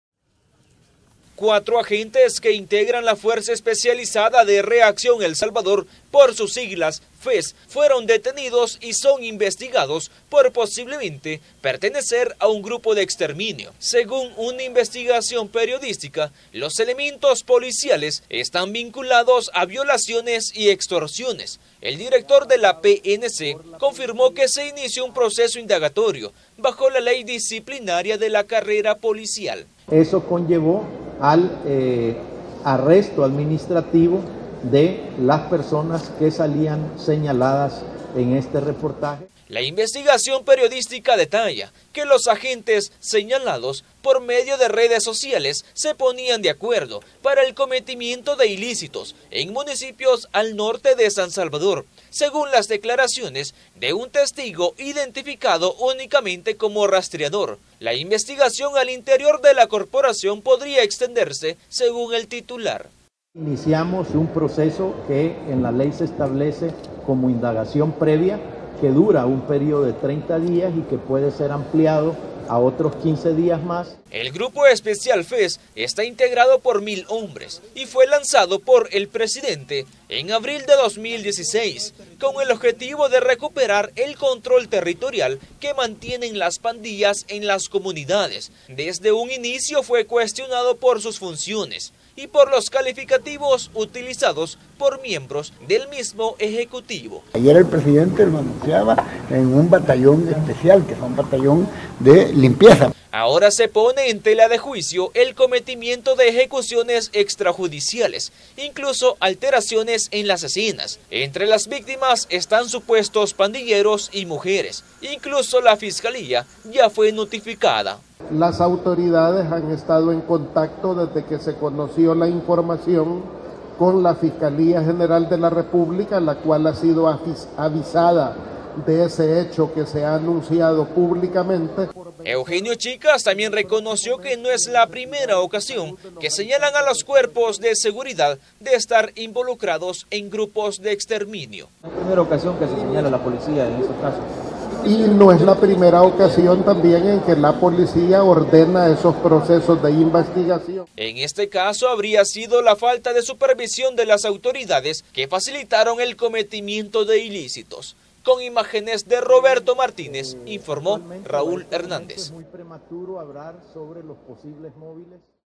CN: Capturan a agentes involucrados en grupos de exterminio
Las autoridades de seguridad revelaron que varios agentes policiales fueron detenidos por estar presuntamente involucrados en un grupo de exterminio que habría cometido ejecuciones extrajudiciales, los policías habrían cometido también otros delitos.